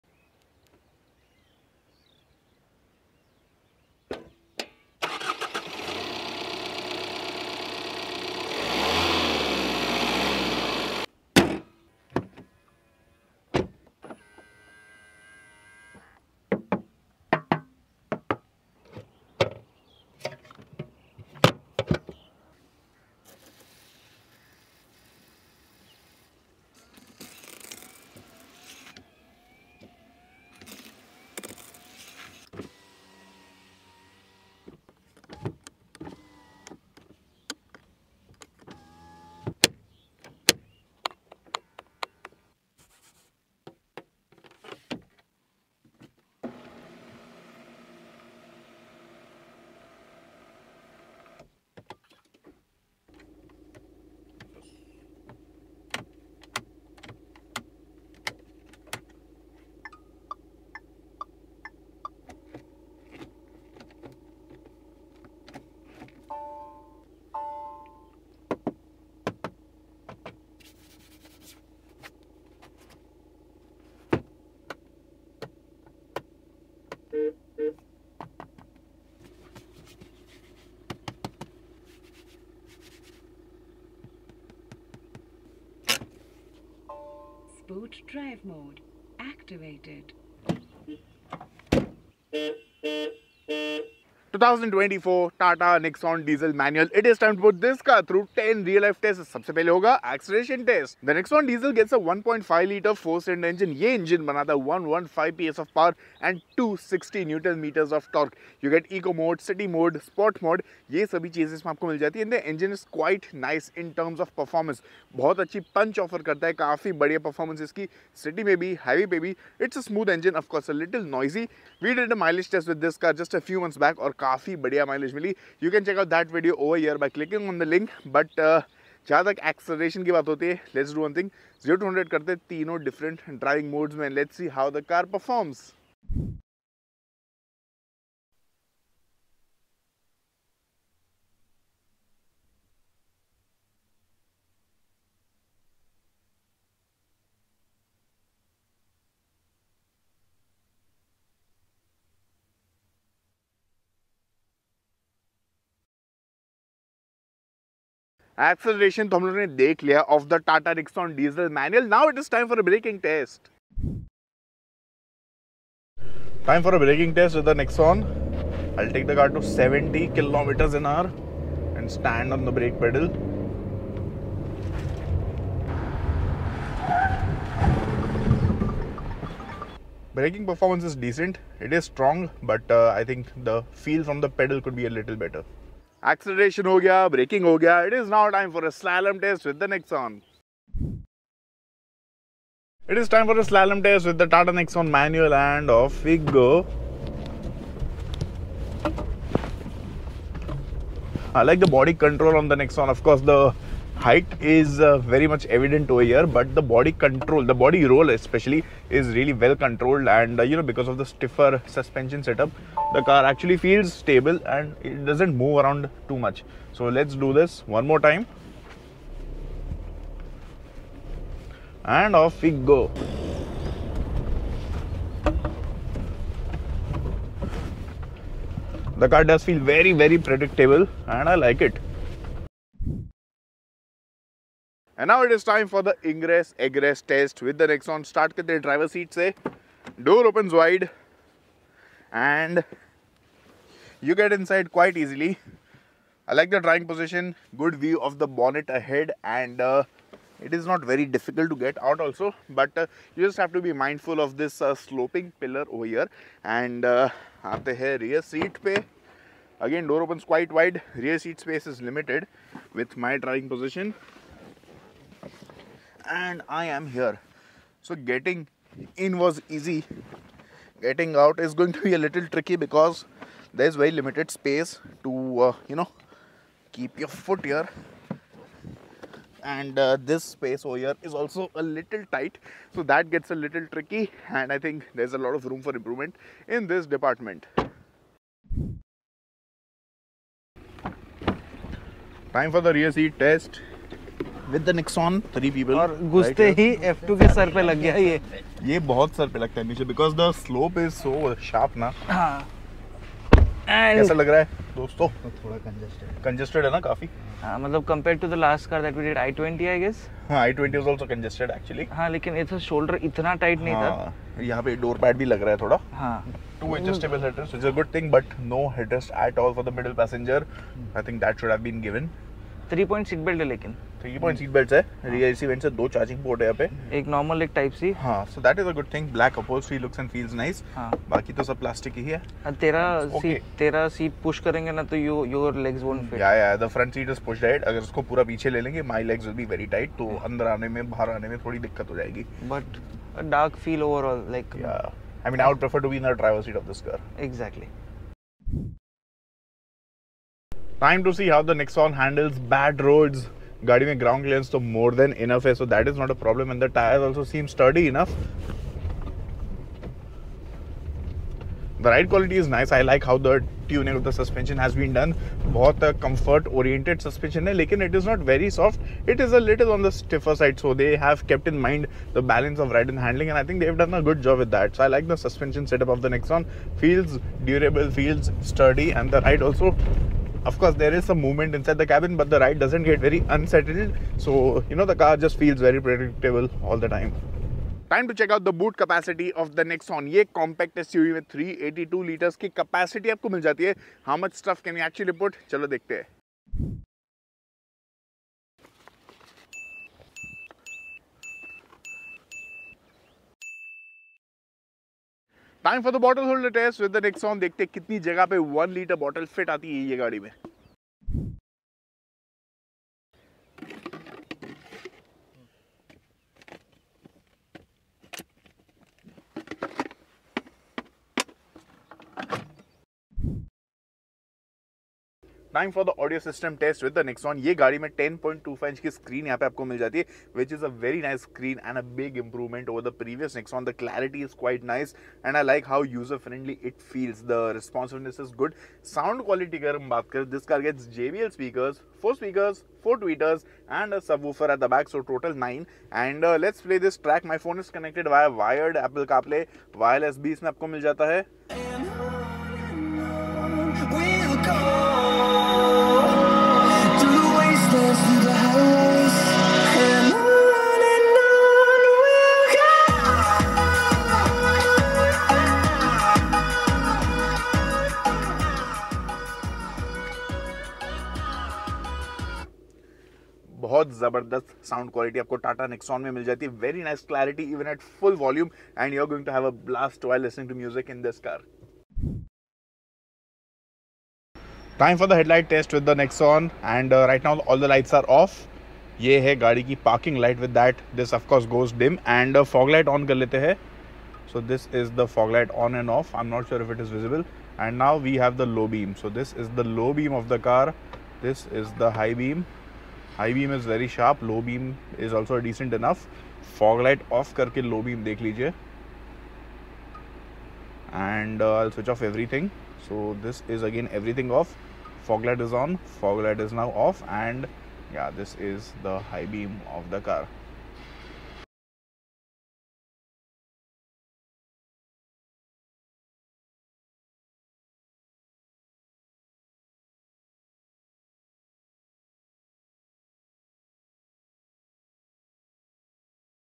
Tata Nexon Diesel MT - 10 Real-Life Tests
In this video we put the 2024 Tata Nexon diesel manual through 10 real-life tests!

Become a MotorBeamer and join the MBSquad

0:00 ASMR
1:34 Introduction
1:41 Acceleration Test
2:45 Braking Test
3:05 Slalom Test
3:55 Ingress Egress Test
4:48 Rear Seat Test
5:49 Bad Road Test
6:28 Boot Space Test
6:52 Bottle Holder Test
7:03 Audio System
8:04 Headlight Test